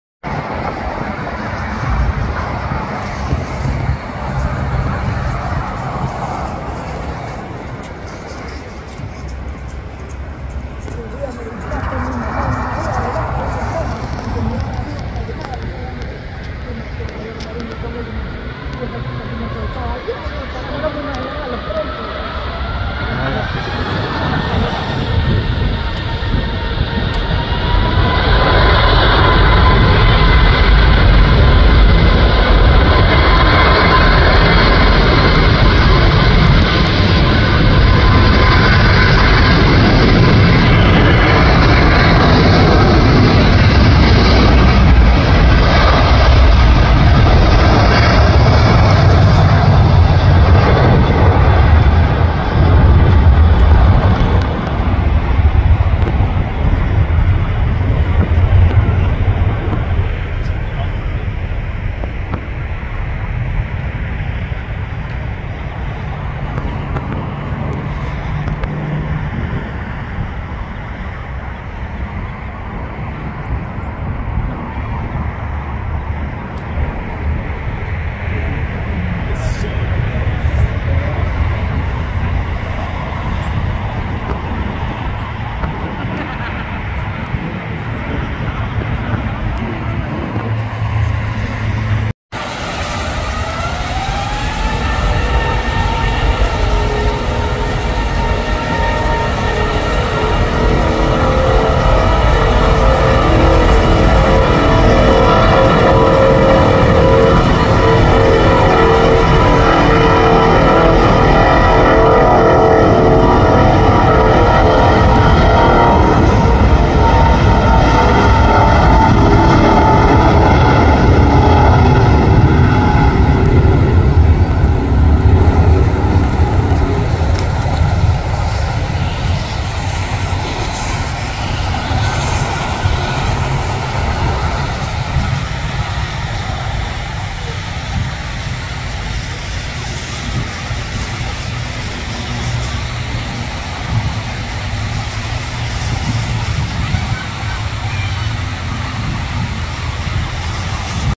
Take Off Between Emirates A380 vs Lufthansa A380 in Reverse Ops at Los Angeles Int'l Airport [4K]
Due to the prevailing winds, aircraft at LAX normally approach and depart to the west (westerly operations); yet sometimes weather requires operations to reverse, with aircraft arriving and departing to the east (easterly operations).

LOCATION OF THIS VIDEO
- Los Angeles International Airport
-Clutter's Park
-El Segundo